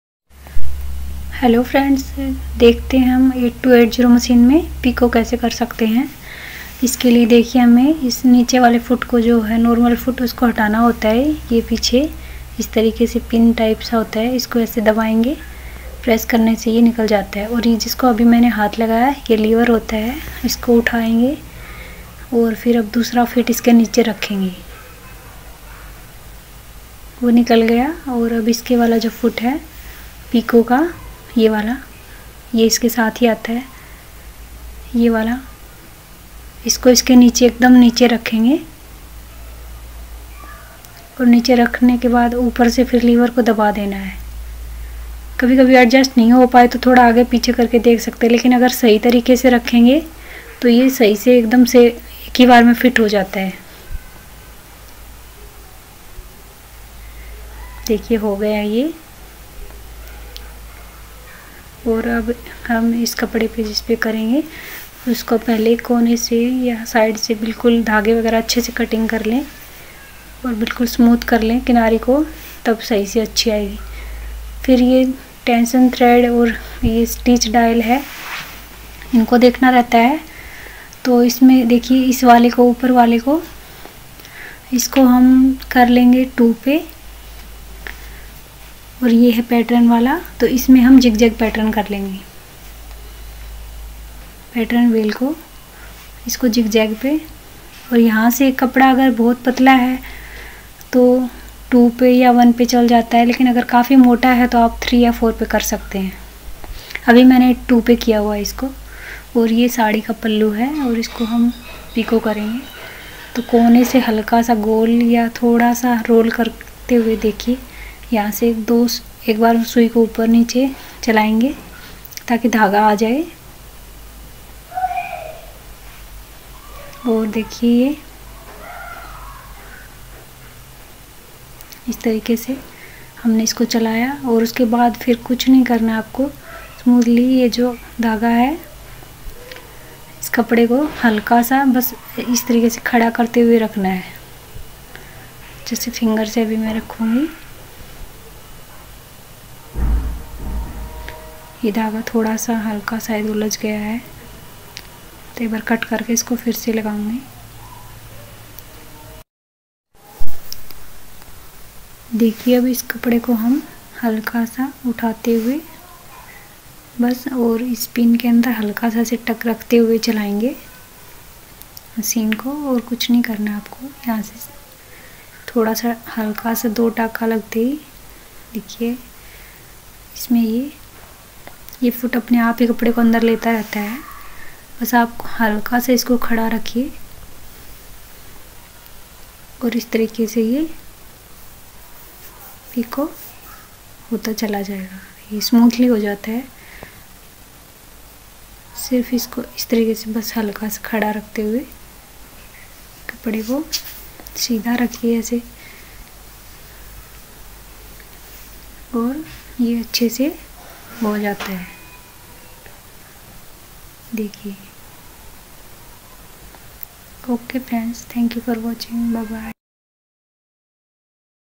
पिको कैसे करे with singer sewing machine 8280
how to do Pico/hem with singer sewing machine 8280 / पिको कैसे करे with singer sewing machine 8280